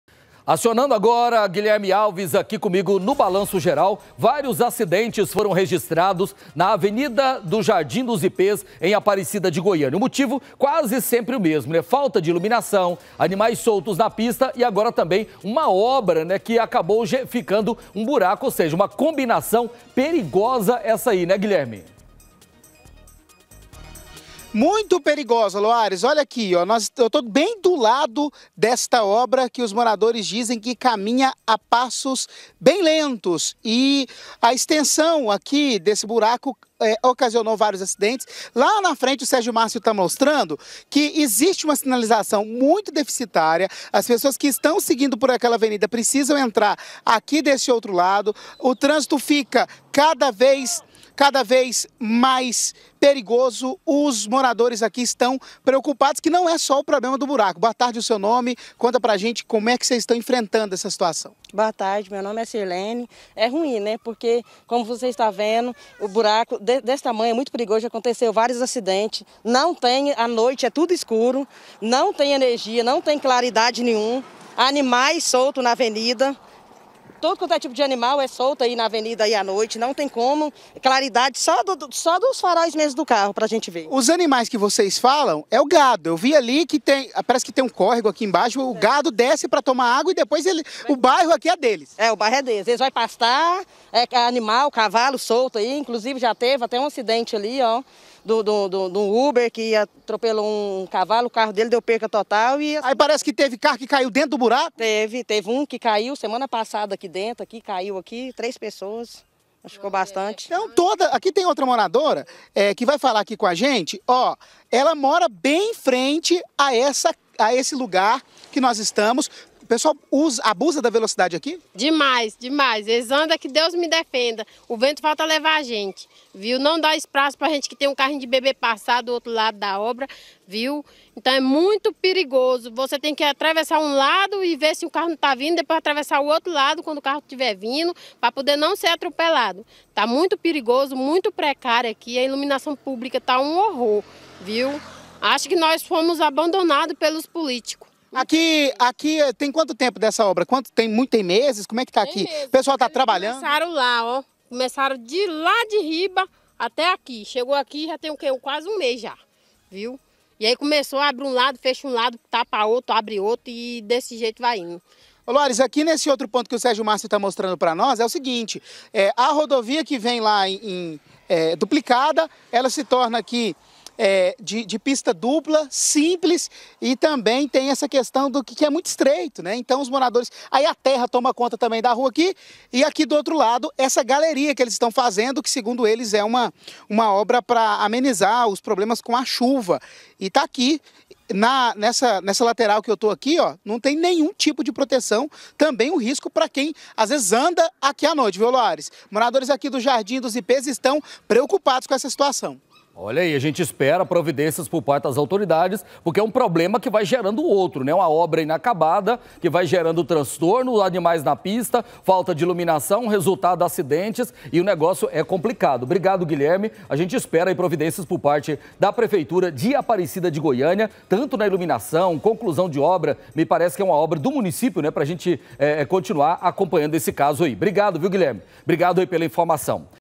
PERIGO! VÁRIOS ACIDENTES NO JARDIM DOS IPÊS
VÁRIOS ACIDENTES FORAM REGISTRADOS NA AVENIDA NO JARDIM DOS IPÊS EM APARECIDA DE GOIÂNIA. E O MOTIVO E QUASE SEMPRE O MESMO. FALTA DE ILUMINAÇÃO, ANIMAIS SOLTOS NA PISTA E AGORA TAMBÉM UMA OBRA QUE FORMOU UM BURACO NA REGIÃO.